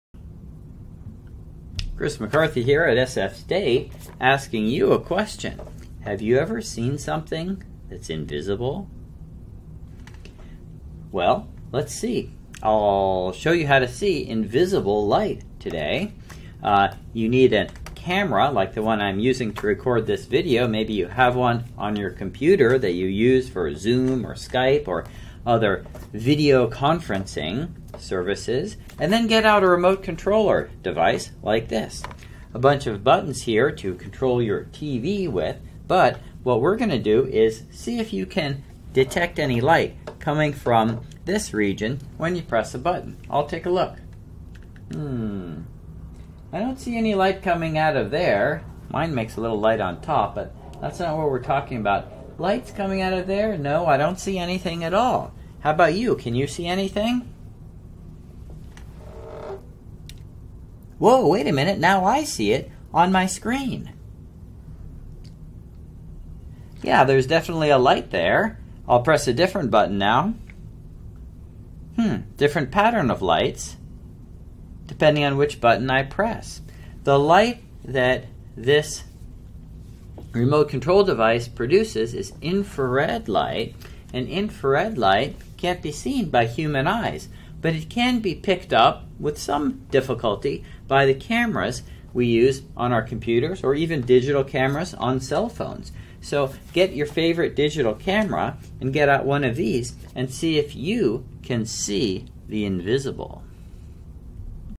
InfraredRemoteControl
Most of us are familiar with remote control handsets.  But how do they work?  Using a camera on your computer, you can actually see infrared light emitted by remote control devices, even though our eyes can't see it!